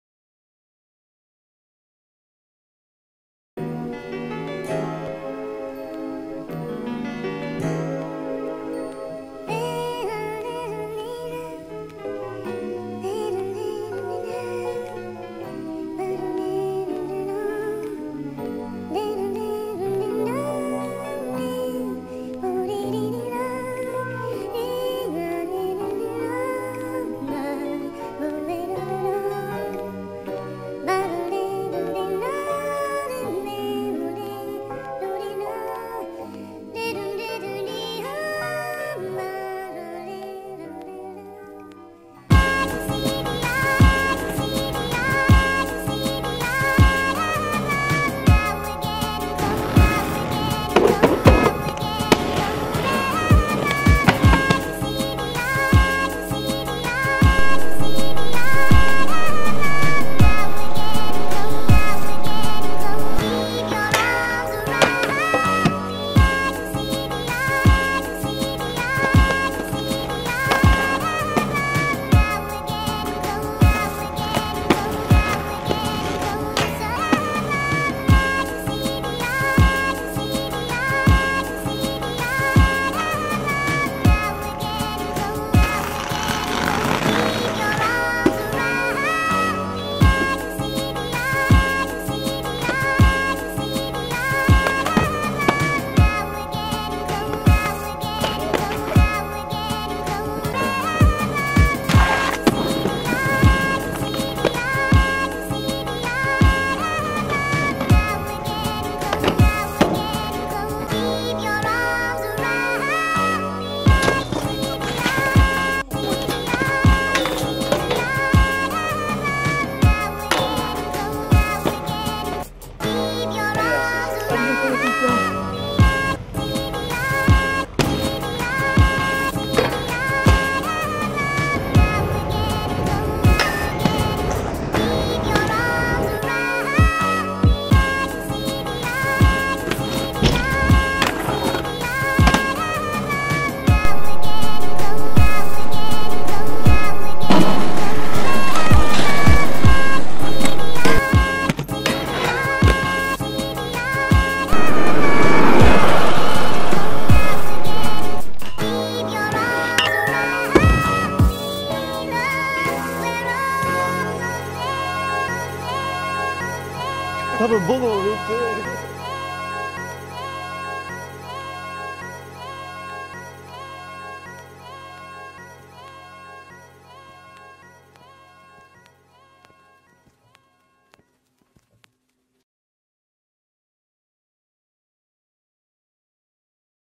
【Color】 versclub ShintaroNakayama InlineSkate
Product by versclub
Feat. Shintaro Nakayama Yuto Akiyama & Danny Abe Ryu
Tracks by Tight Cg
Edit by Shintaro Nakayama